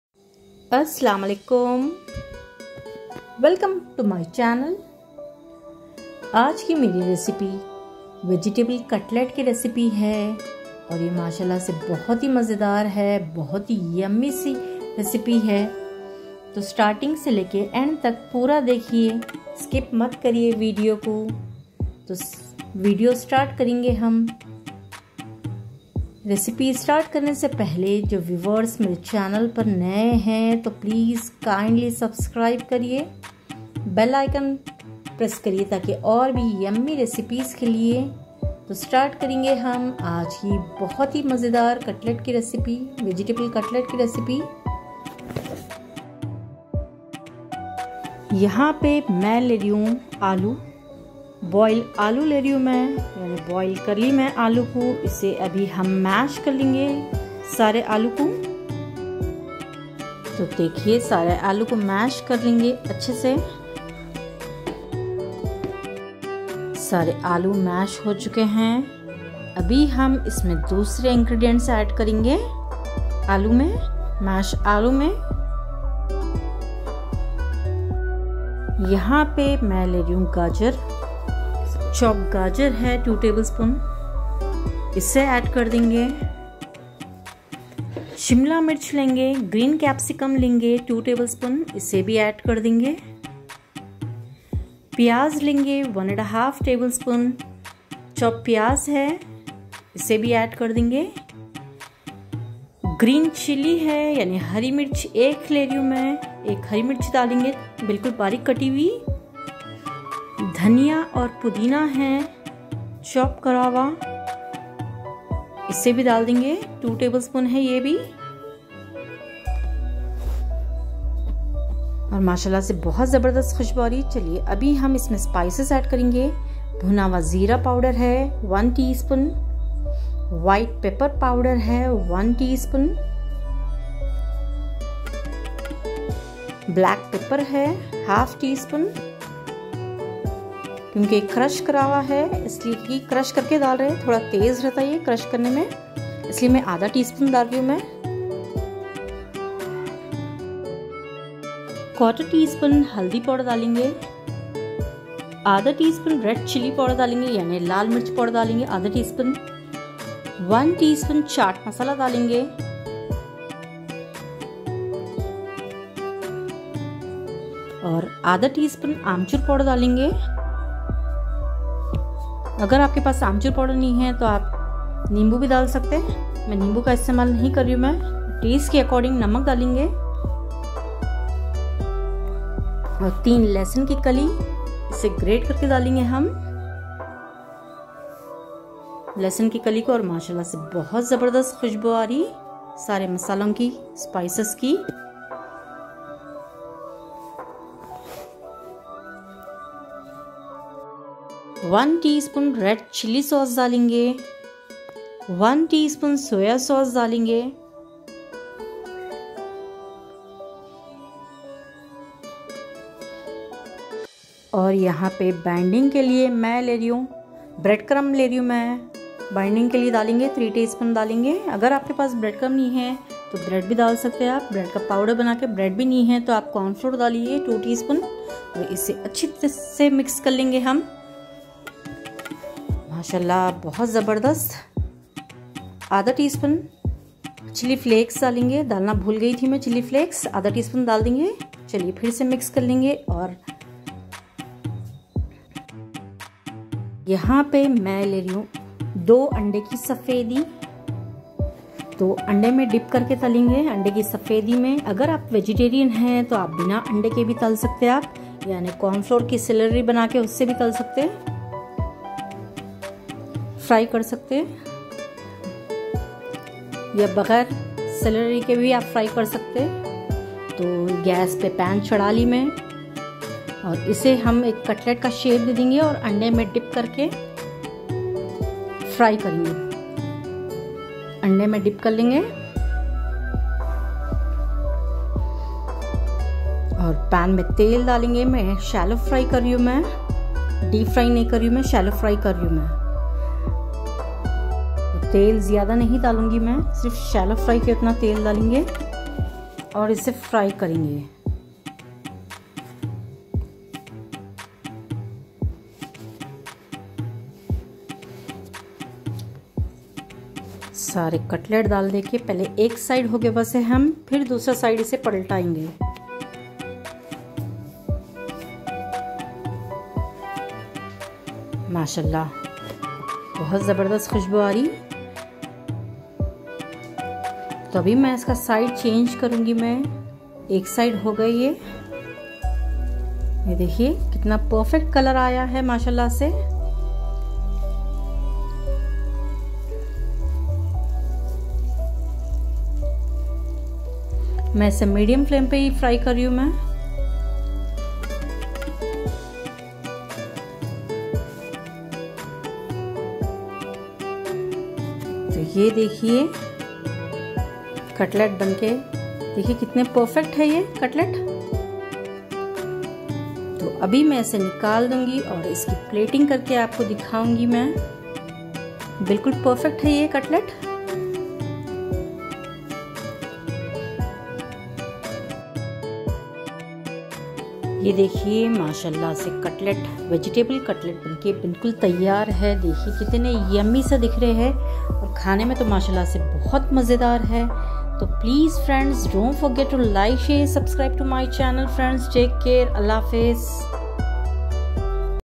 Vegetable Cutlet Recipe// Cutlet Recipe// Cook with Nafa
ingredients:

boil potatoes  🥔 5
chop carrot 🥕 2 tbls
chop green capsicum  2 tbl
chop Onion 🌰 1 1/2  tbls
green chilli 🌶 1
chop mint n coriander leaves 2 tbls
garlic cloves 3
roasted cumin powder 1 teaspoon
chilli flakes 1/2 teaspoon
white pepper powder 1 teaspoon
black pepper powder 1/2 teaspoon
bread crums  🍞 3 teaspoon
chat masala 1 teaspoon
dry mango powder 🥭 1/2 teaspoon
red chilli sauce 1 teaspoon

Hope u guys enjoyed my video by watching 😍❤